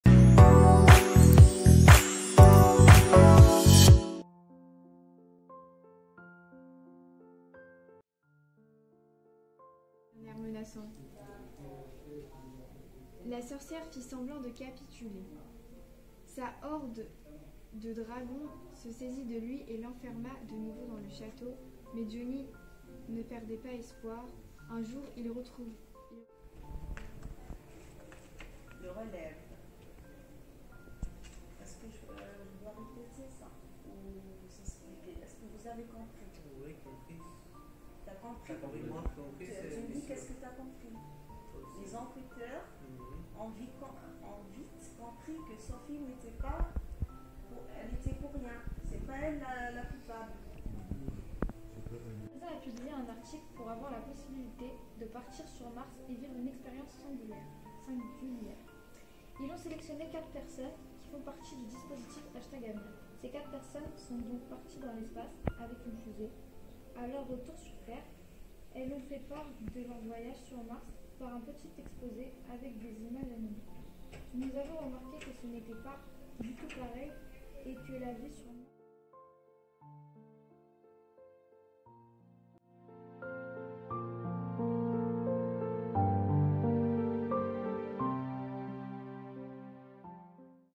Atelier de Lecture partagé
Les apprenants de Avenir (Région Normandie) ont animé une séance de lecture au près du public Savoirs Langue Française (Région Normandie) à Saint-Etienne du Rouvray.
L'objectif: Faire parler les apprenants (production orale), enrichir leur vocabulaire en français à partir d'un livre qui a été rédigé par d'anciens apprenants de Avenir.